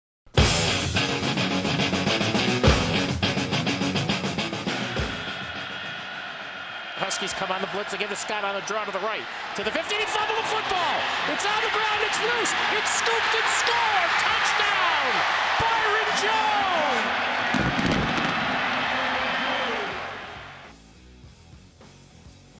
15 Years at Rentschler Field - 2011 Fumble Recovery vs. USF
Rentschler15 Highlights

2011: Freshman safety Byron Jones picked up a fumble forced by Twyon Martin and scored from 10 yards out midway in the 3rd quarter to lead UConn Football to a 16-10 Homecoming win over USF.